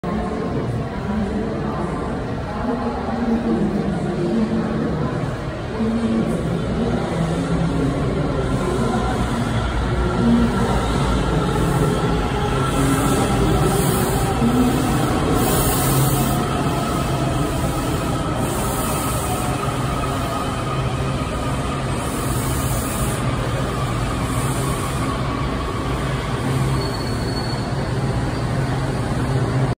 WMATA Metrorail Kawasaki 7000 Series On The Red Line At Metro Center Station
Washington Metropolitan Area Transit Authority Metrorail Kawasaki 7000 Series on the Red Line arriving at Metro Center Station en route to Shady Grove Station which is one of the end points of the Red Line.